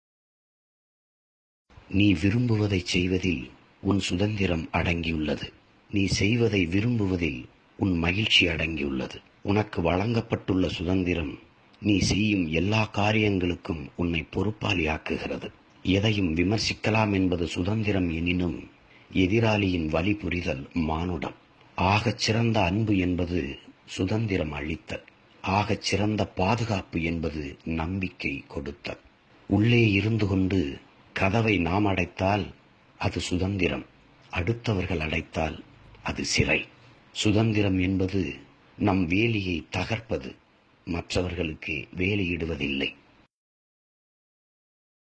17-08-2017 தினம் ஒரு செய்தி - 65 - சீமான் தலைமை ஒருங்கிணைப்பாளர் நாம் தமிழர் கட்சி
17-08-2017 தினம் ஒரு செய்தி - 65 | பகுதி - 02

நீ விரும்புவதைச் செய்வதில் உன் சுதந்திரம் அடங்கியுள்ளது.
நீ செய்வதை விரும்புவதில் உன் மகிழ்ச்சி அடங்கியுள்ளது.

உனக்கு வழங்கப்பட்டுள்ள சுதந்திரம் நீ செய்யும் எல்லாக் காரியங்களுக்கும் உன்னைப் பொறுப்பாளியாக்குகிறது.

எதையும் விமர்சிக்கலாம் என்பது சுதந்திரம் எனினும், எதிராளியின் வலிபுரிதல் மானுடம்.

ஆகச் சிறந்த அன்பு என்பது சுதந்திரம் அளித்தல்.
ஆகச் சிறந்த பாதுகாப்பு என்பது நம்பிக்கை கொடுத்தல்.

உள்ளே இருந்து கொண்டு கதவை நாம் அடைத்தால் அது சுதந்திரம்; அடுத்தவர்கள் அடைத்தால் அது சிறை!

சுதந்திரம் என்பது நம் வேலியைத் தகர்ப்பது மற்றவர்களுக்கு வேலியிடுவதில்லை.

சீமான்
தலைமை ஒருங்கிணைப்பாளர்